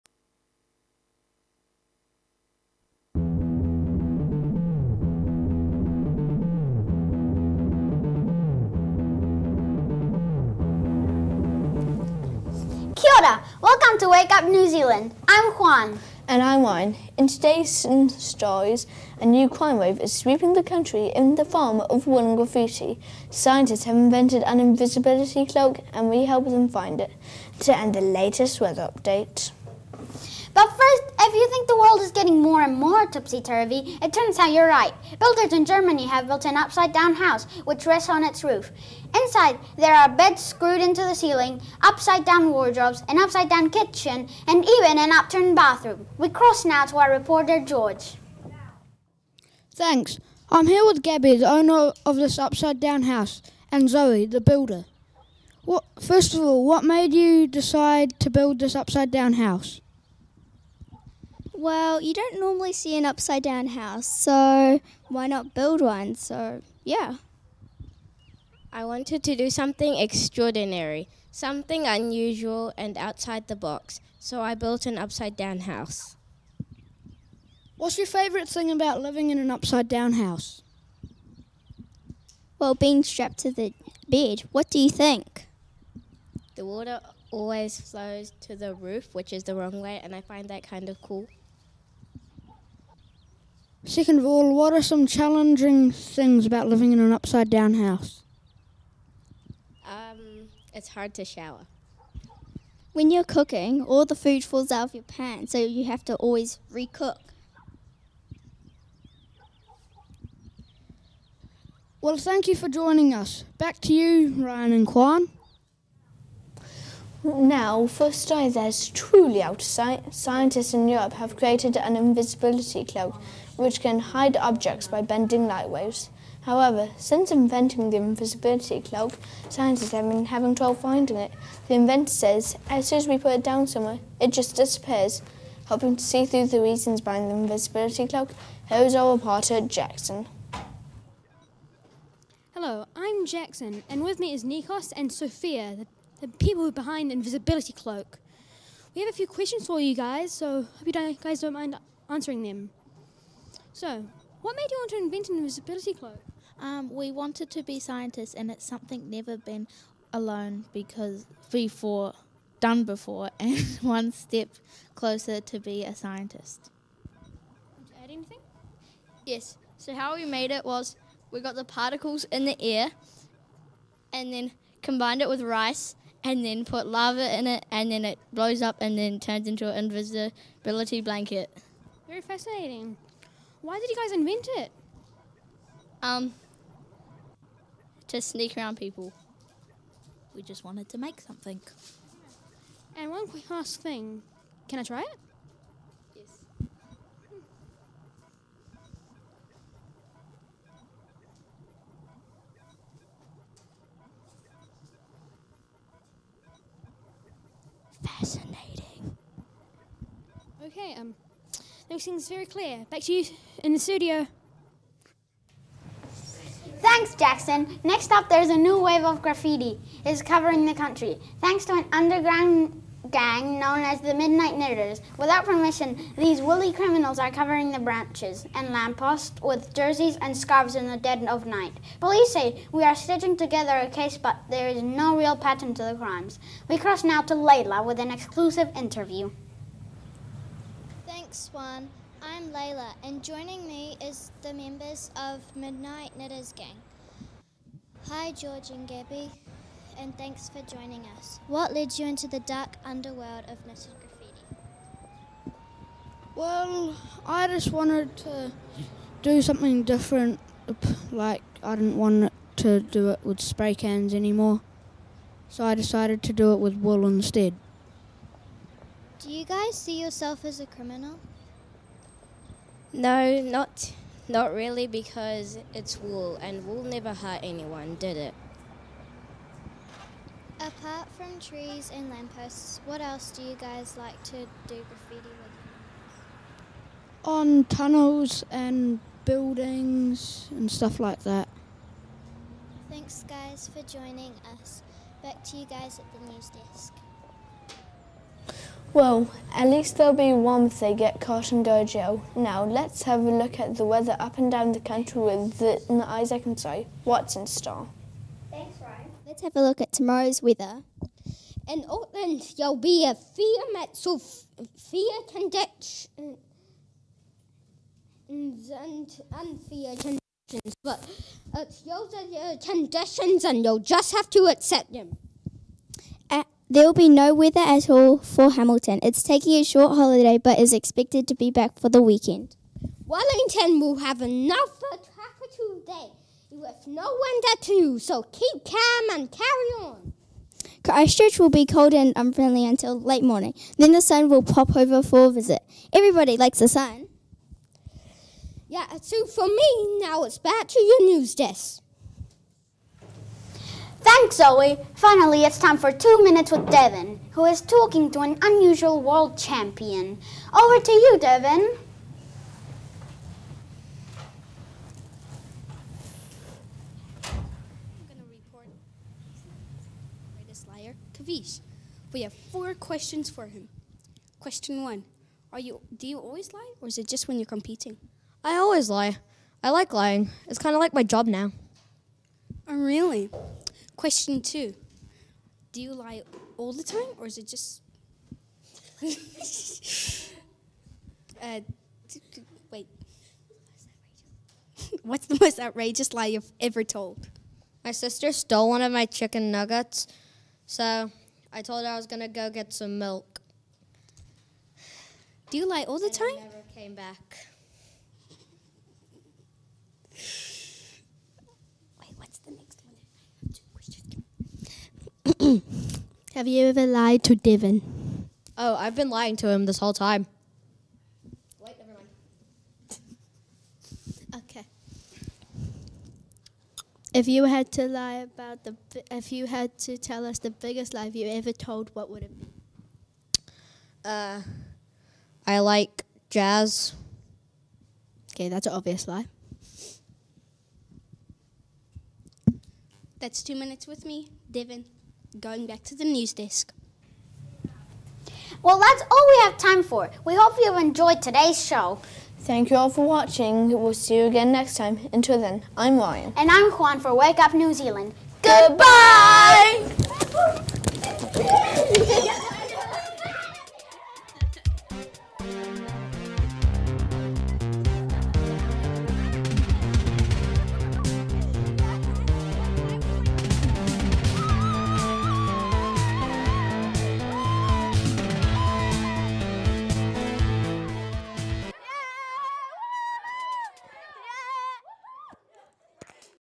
Eastern Hutt G1 presents WAKE UP NZ!
Wake Up New Zealand, it's Eastern Hutt School!

Join the crew for a grab bag of random headlines from across the globe. We take a peep inside an upside down house, science goes out of sight - literally - with the invention of an invisibility cloak, then we hit the streets for the low-down on yarn bombing with two of the notorious Midnight Knittaz. All that plus your weather forecast for tomorrow. We hope you enjoy watching! Recorded live in the Capital E OnTV Studio, Wellington on Thursday 27th October 2022.